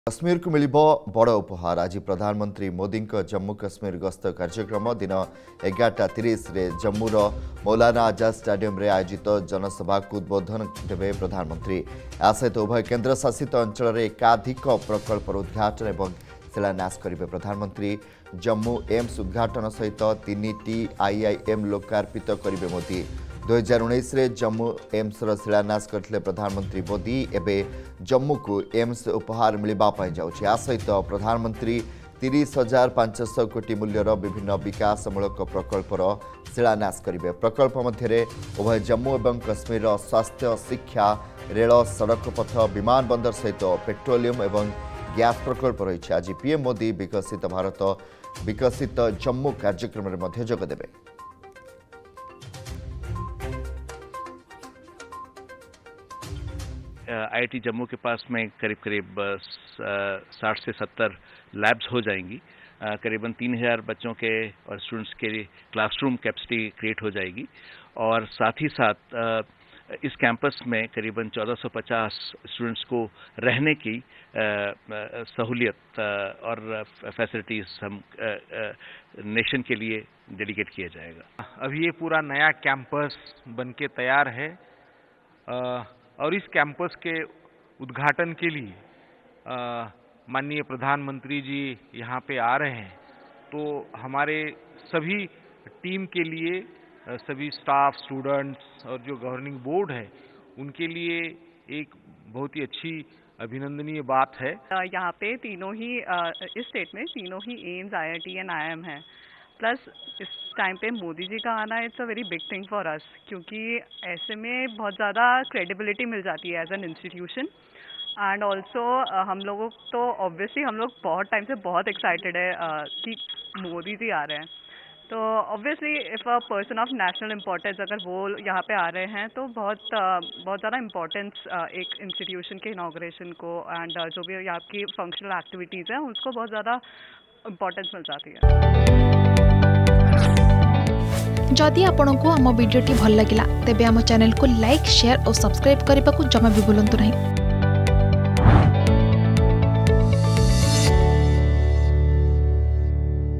ଆଜି କାଶ୍ମୀରକୁ ମିଳିବ ମୋଦିଙ୍କ ବଡ଼ ଉପହାର || Narendra Modi
ପ୍ରଧାନମନ୍ତ୍ରୀ ମୋଦିଙ୍କ ଜାମ୍ମୁ ଓ କାଶ୍ମୀର ଗସ୍ତ । ଜାମ୍ମୁ ଏମ୍ସ ସହ 3ଟି IIM ଲୋକାର୍ପିତ କରିବେ ମୋଦି ।